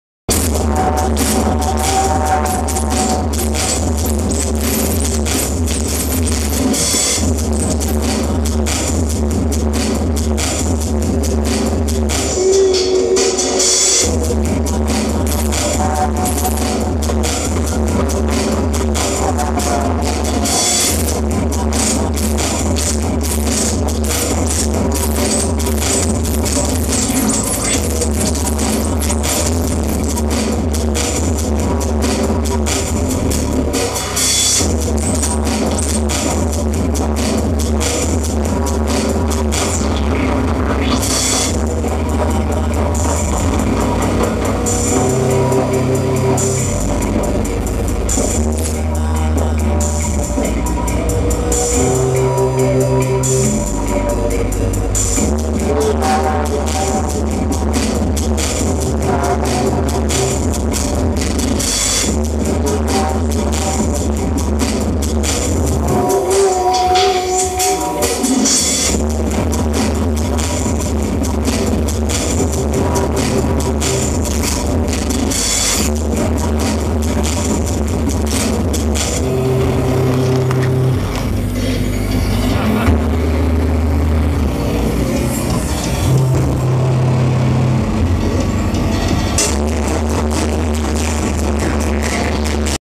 PSK live at The Spirit of Gravity Feb 2017
PSK live at The Spirit of Gravity Feb 2017 second section.
Kev Hough, Steve Fagan, Pat ?
The Worthing supergroup.
Including comedy bass wobbling the camera off the wall moment.